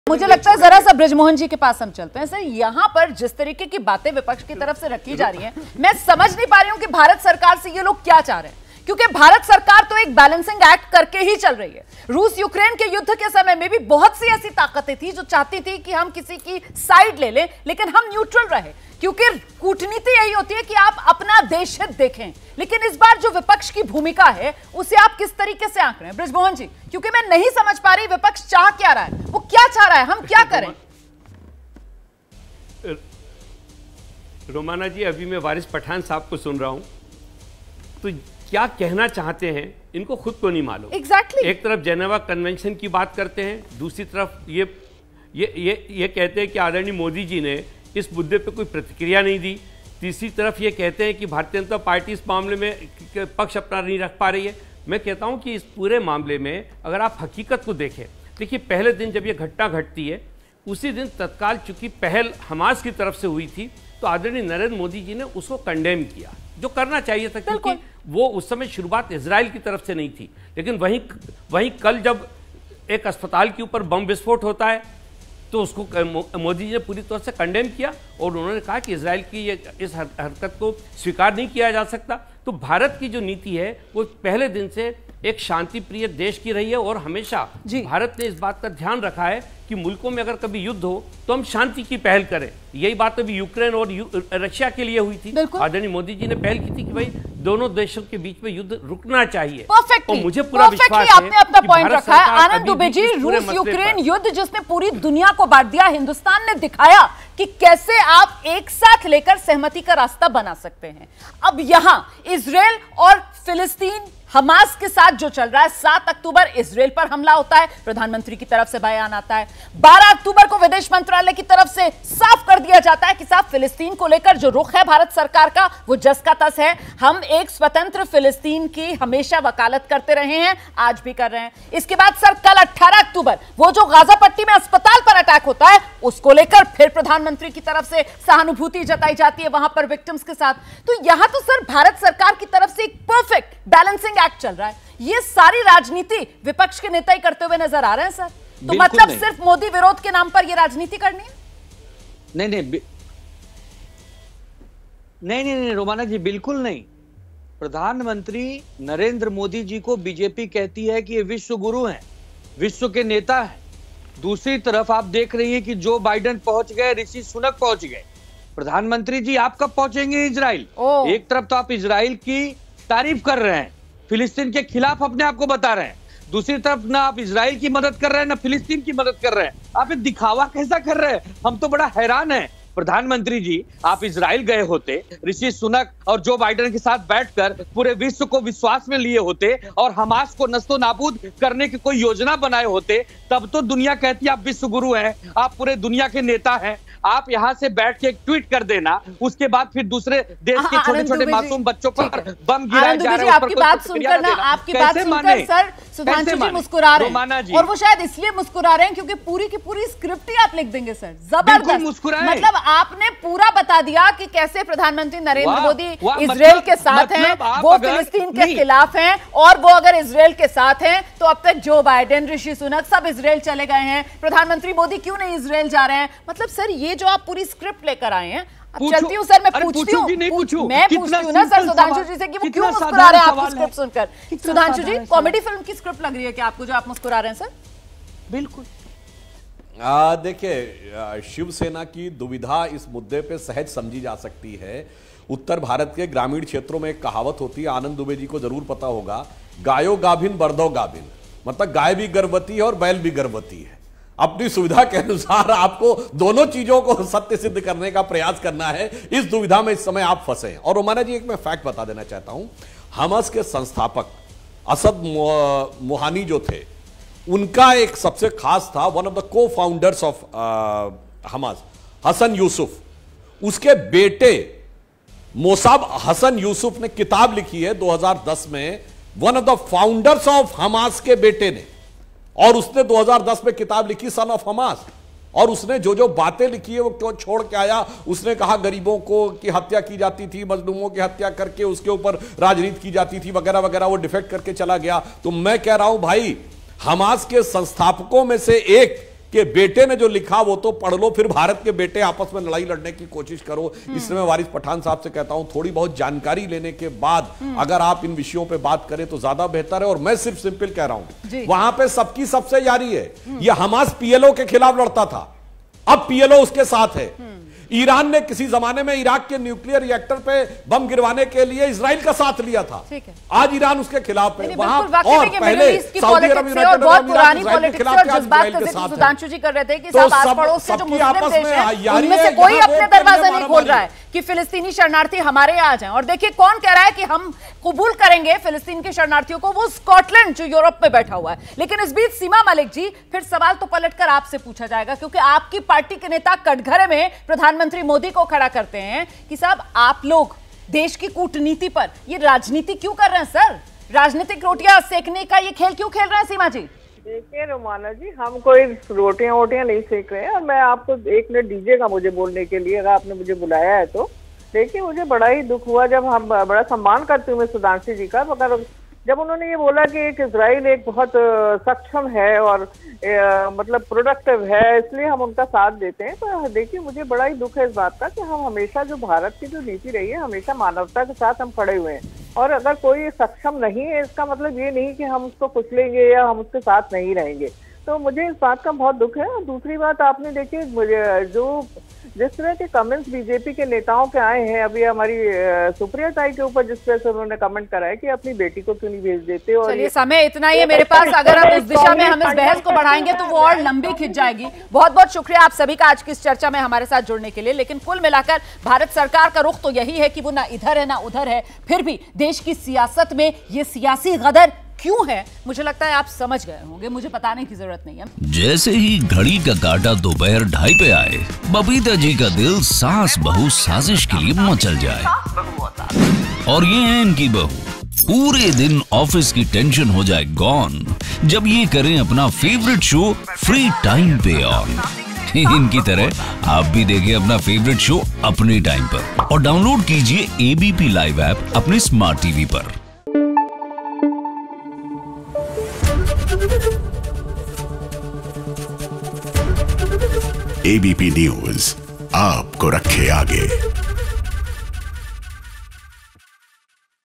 Israel Hamas War: आखिर विपक्ष इजरायल हमास युद्ध पर मोदी सरकार से क्या चाहता है ?
मुंबई स्थित क्षेत्रीय कार्यालय में रविवार (15 अक्टूबर) को पदाधिकारियों की बैठक हुई. इस दौरान एनसीपी अध्यक्ष शरद पवार ने इजरायल और फलस्तीन के बीच हो रही जंग पर अपनी प्रतिक्रिया दी. उन्होंने इस बैठक में फलस्तीन के प्रति अपना समर्थन दिखाया. उन्होंने कहा, ''इजरायल-फलस्तीन की जंग विश्व शांति के लिए खतरा है. वहां के जमीन और घर फलस्तीन के थे, इजरायल का उस पर कब्जा हो गया." शरद पवार के बयान को लेकर केंद्रीय मंत्री पीयूष गोयल ने पलटवार किया है. पूर्व मुख्यमंत्री शरद पवार की प्रधानमंत्री नरेंद्र मोदी पर की गई टिप्पणी पर गोयल ने कहा कि उन्हें पहले देश के बारे में सोचना चाहिए.